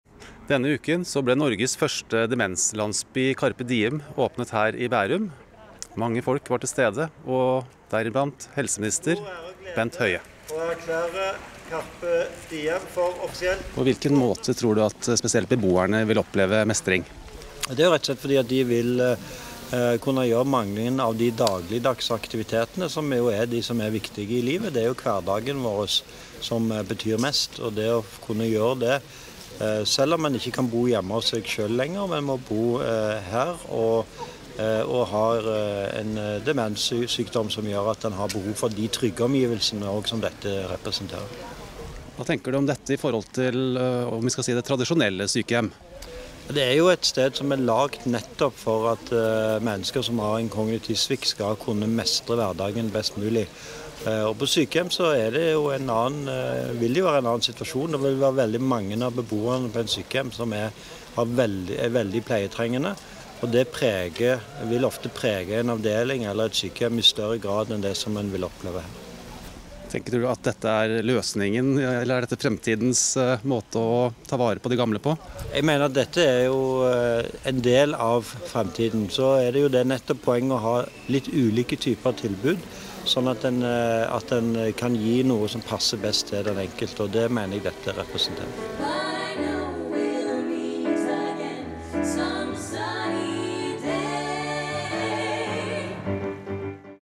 Helse-og omsorgsministeren om Norges første demenslandsby
Bent Høie sier demenslandsbyen Carpe Diem i Bærum er en positiv tilvekst til eldreomsorgen i Norge. Den vil gi mennesker med demens større mulighet til å mestre sin egen hverdag til tross for sykdommen, mener han.